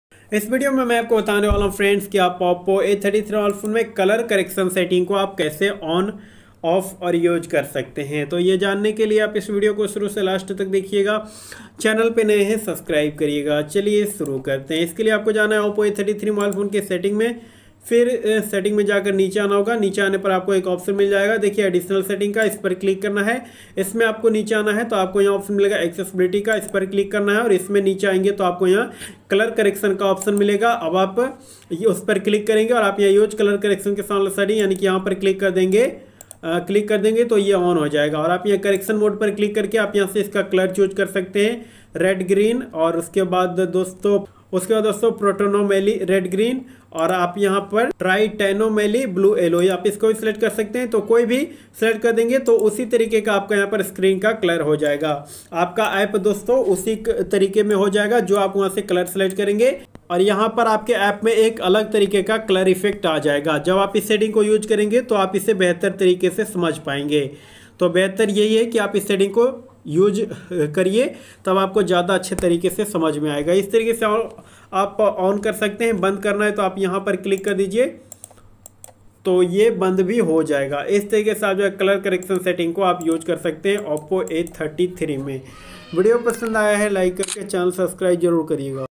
Oppo A33 me color correction on/off kaise kare ।। how to use color correction in oppo a33
is video me bataya gaya hai friends ki oppo A33 mobile me color correction setting ko on karke use kaise kiya jata hai